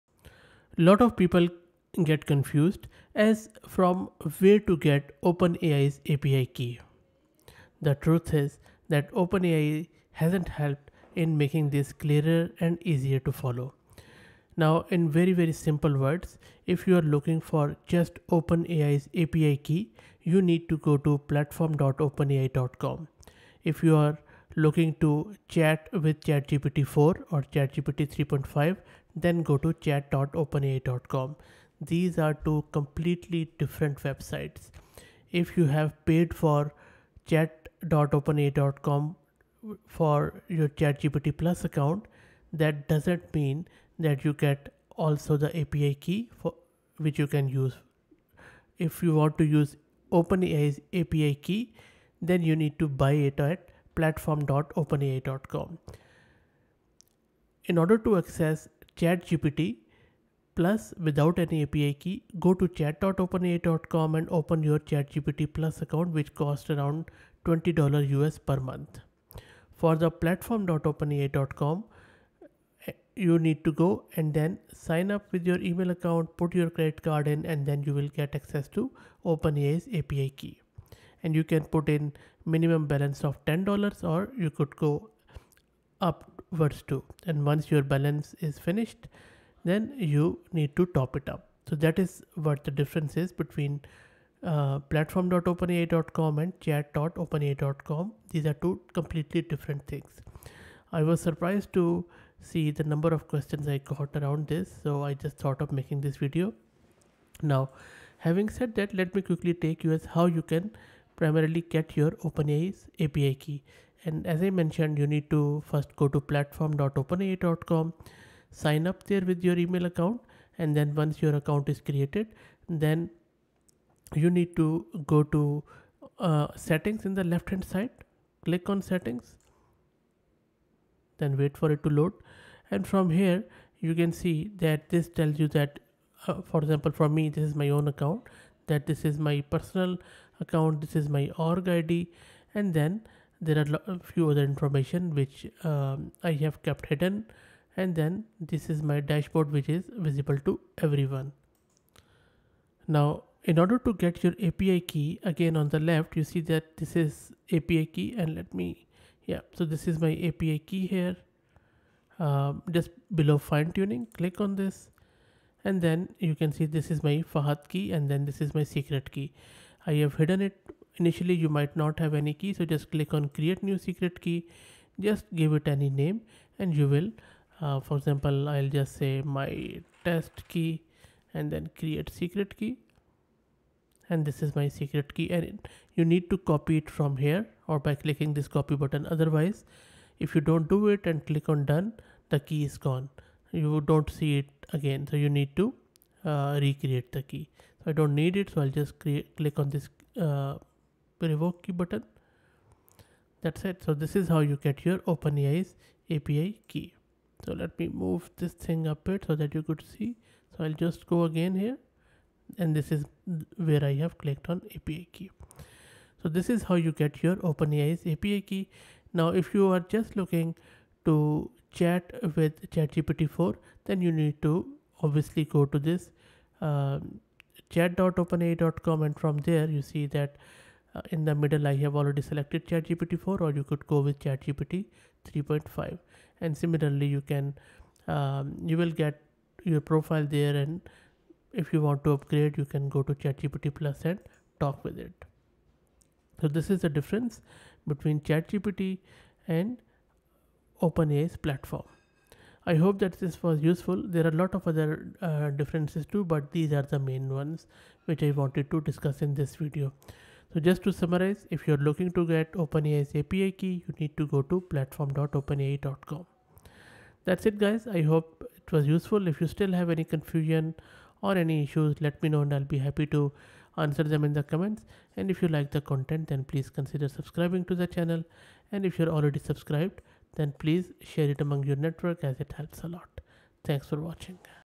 Understanding OpenAI Platforms - Where to Get Your API Key and Chat with ChatGPT
This video describes difference between chatgpt and openai platform and how to get OPEN AI Key easily.